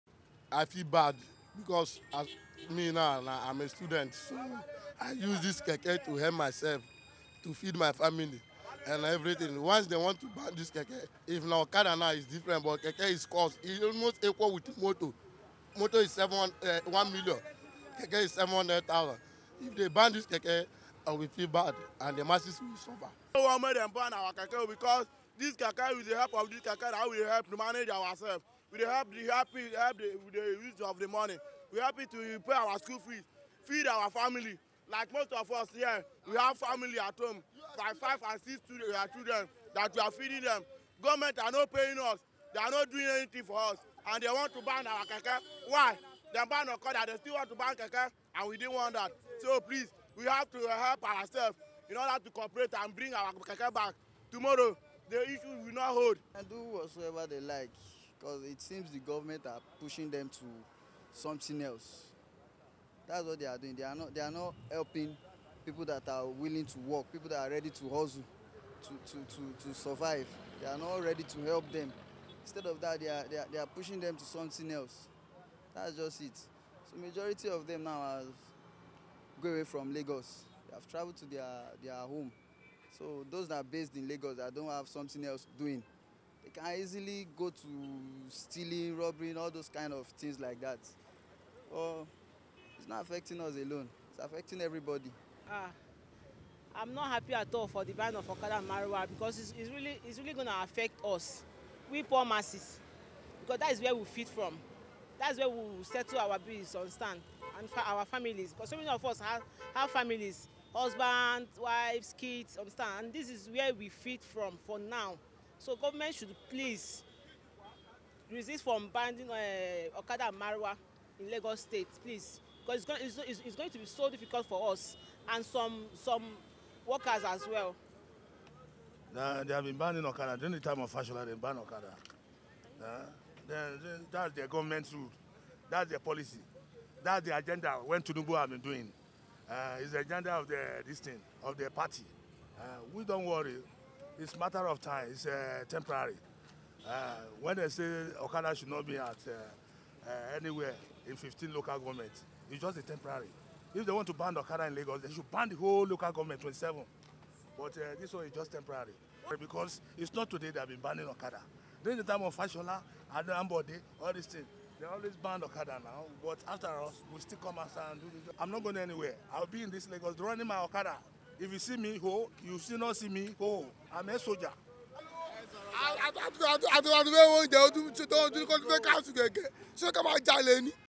Keke Drivers & Okada Riders Express Their Worries On The Ban Commencing Feb. 1
The Lagos State government has decried the rate of motorcycle (okada) and tricycle (keke) accidents on roads across the state.